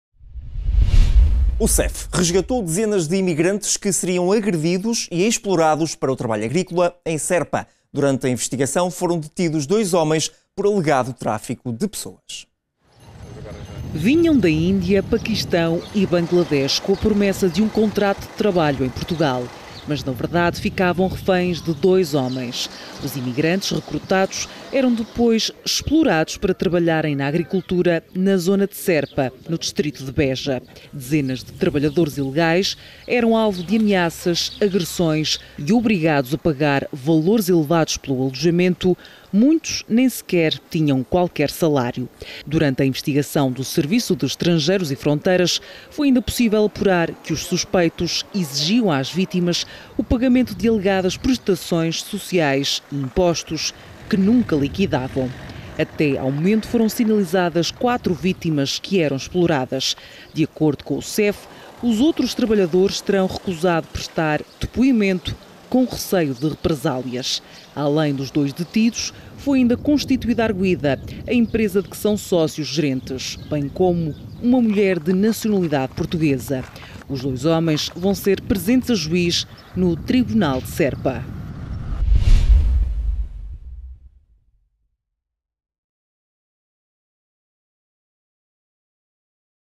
Exploração laboral em Serpa
O SEF resgatou dezenas de imigrantes que terão sido agredidos e explorados para o trabalho agrícola, em Serpa. Durante a investigação, foram detidos dois homens por alegado tráfico de pessoas.